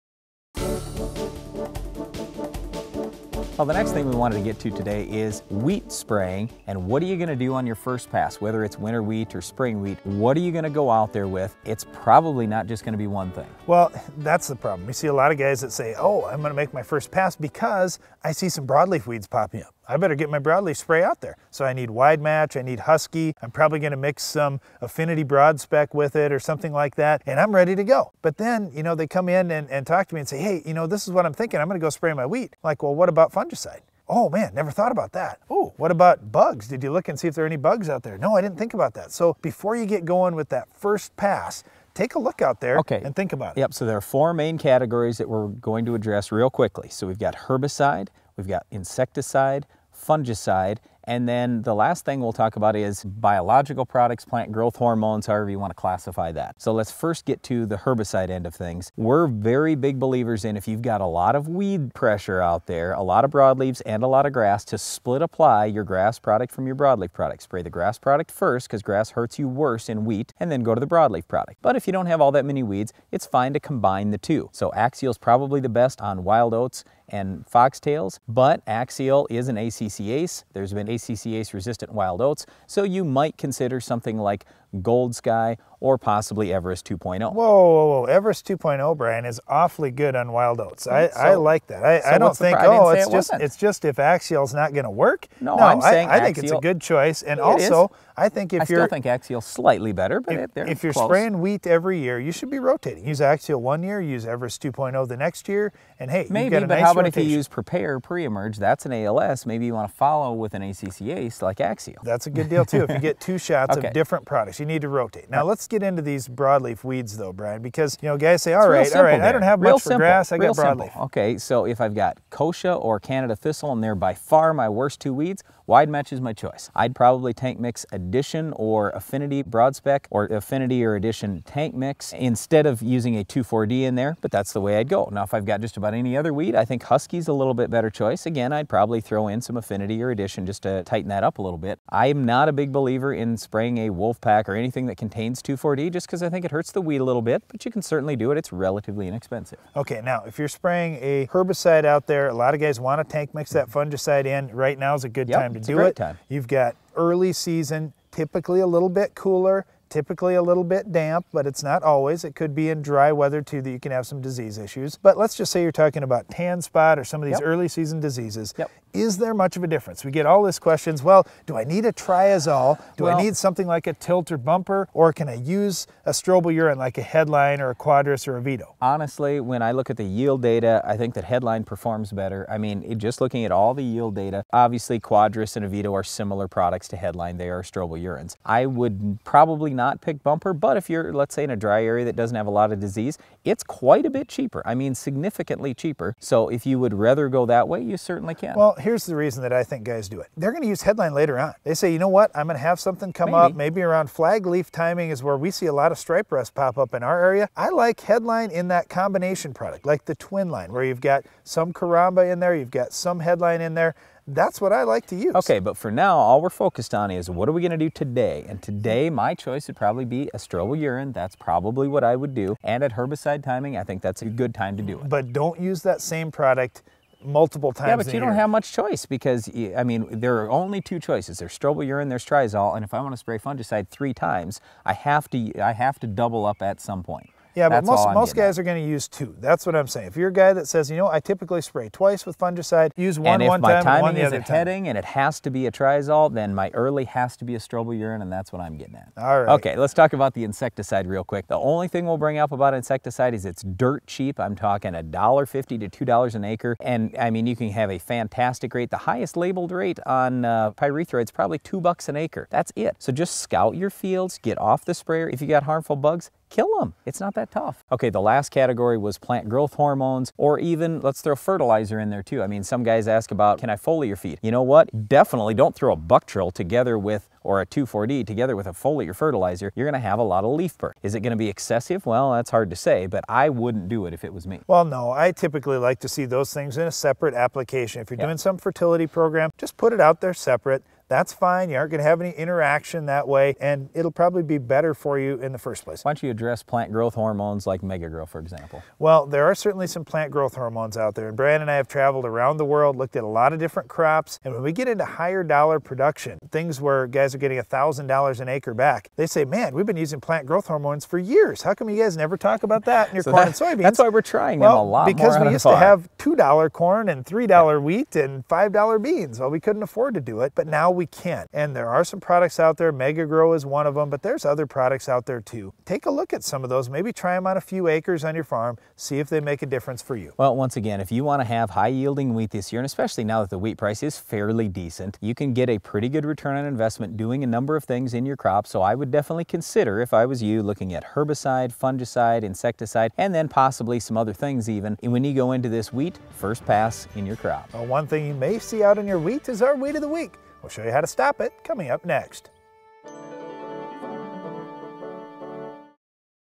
Spraying Wheat #731 (Air Date 4/8/12)
Brian & Darren go over when to be spraying your wheat crop.